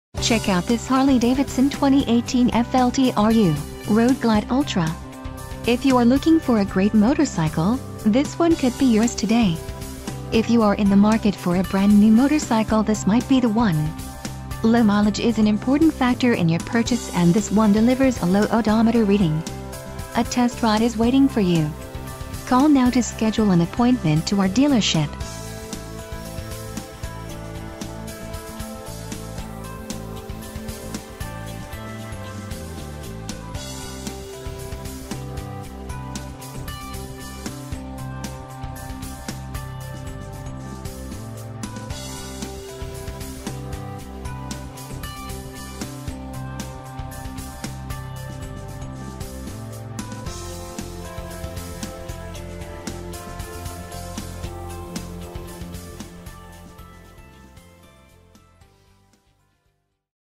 2018 Harley-Davidson FLTRU - Road Glide Ultra - Motor Ci...
Join the Motor City Family Today!2018 Harley-Davidson Road Glide UltraThe ultimate mile-eater for the rider who knows nothings out of bounds.Features may include:Frame-Mounted Sharknose FairingA distinctive, menacing, mile-hungry design that cuts through the wind like no other fairing in the world.Custom WindscreenA thirteen and a half inch custom style windscreen that sits between you and the worlds flying insect population.Dual Daymaker Reflector LED HeadlampsDarkness-defying headlight that ensures youll have no trouble seeing everything out there that needs to be seen.Milwaukee-Eight V-Twin EngineA powerful, cool-running engine with crisp throttle response and a pure, soul-satisfying rumble.Responsive SuspensionHigh-performing front and rear suspension with easily adjustable rear shocks put you in control of a plush ride.Reflex Linked Brembo Brakes with Standard ABSThe front and rear brakes ... read more